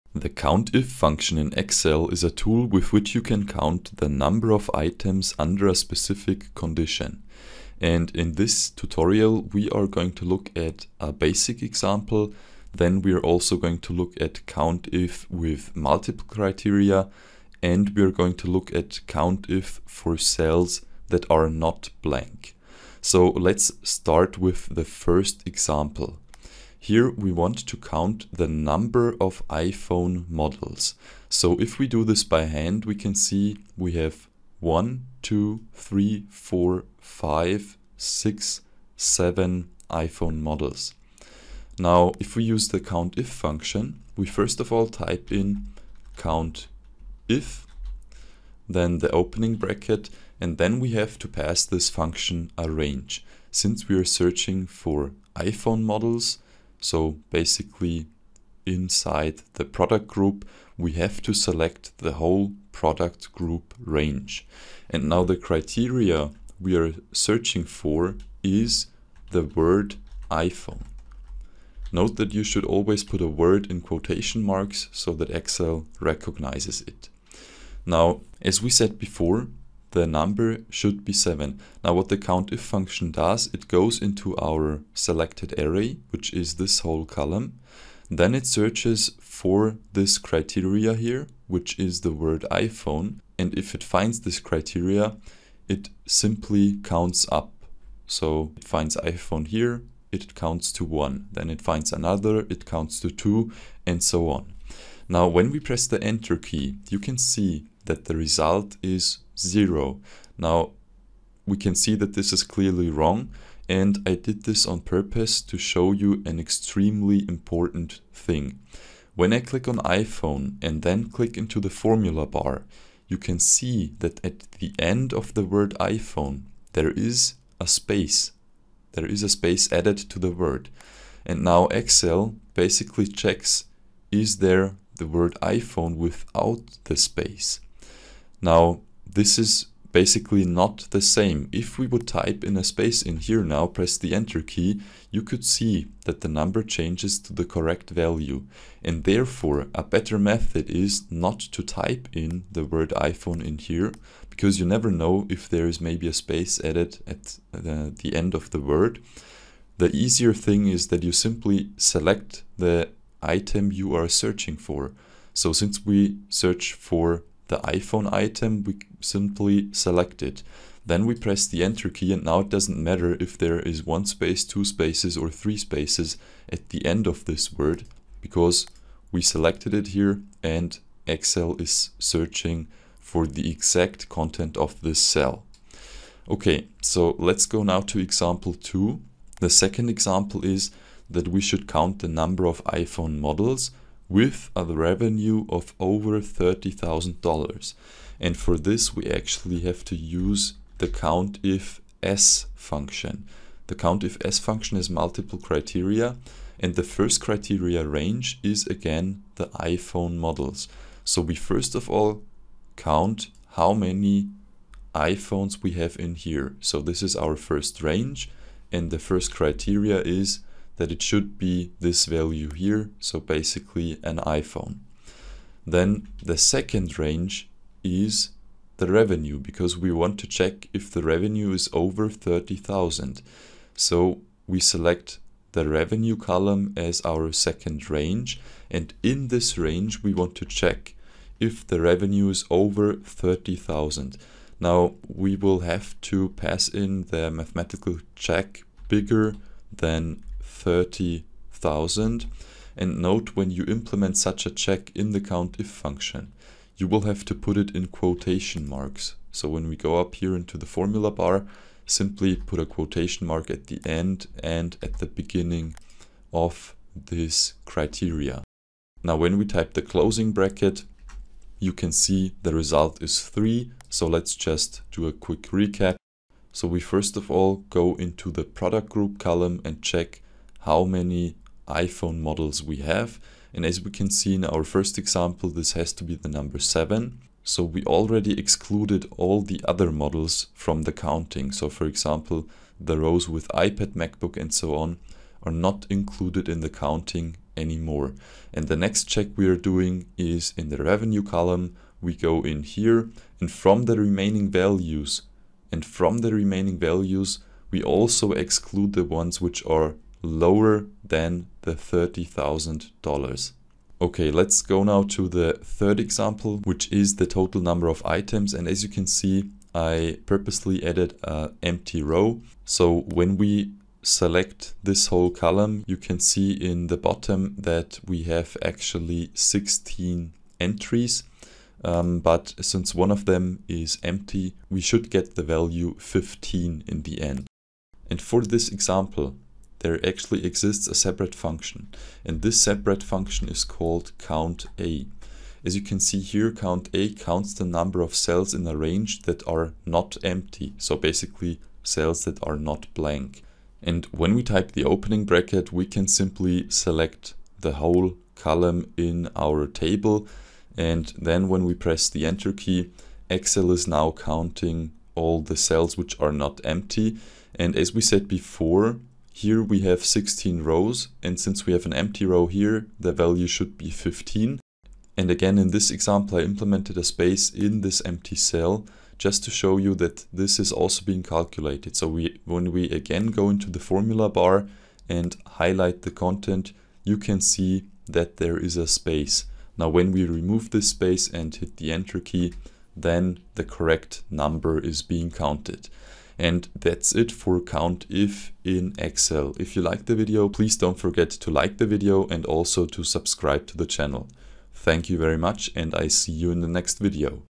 COUNTIF function in Excel ➕ not blank / less than / greater than / multiple criteria
The COUNTIF function in Excel is a tool with which you can count the number of cells which fulfil a certain condition. You can for example do a COUNTIF not blank but also use this formula with mathematical operations like COUNTIF less than or COUNTIF greater than. And sure enough you can also go for the exact match of a number. So, in this video you will learn how to use the COUNTIF function in Excel.

In the first example of this tutorial, we are going to look at a basic example for this Excel formula. In the second example, we will work with multiple criteria in the COUNTIF function. So for this there exists the COUNTIFS function.
And last but not least we are also going to look at COUNTIF not blank.

Timeline:
00:00 Introduction
00:28 COUNTIF with single criteria
01:45 Possible sources of errors
03:08 COUNTIFS with multiple criteria
05:15 COUNT IF not blank